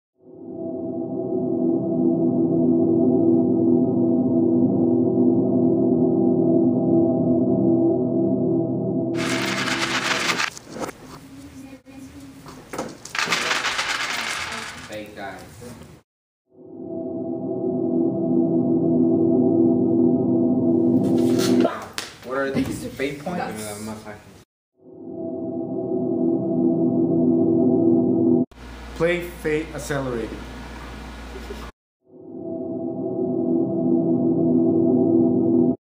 MNTHS: "Fate Accelerated Edition Commercial" by Ereyhon
The students in Mr. Simpson's Introduction To Digital Media Literacy Class played "Fate Accelerated Edition" by Evil Hat Productions, and created a series of videos to document their experience.

In this video, Ereyhon has created a commercial for Fate Accelerated Edition.